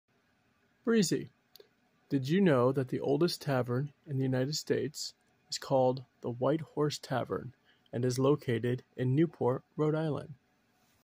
Did you know about the oldest tavern in the United States?
his name is breezy and he's 19 years young. the oldest tavern was built in 1673 in Newport Rhode Island. @Bucketlistbars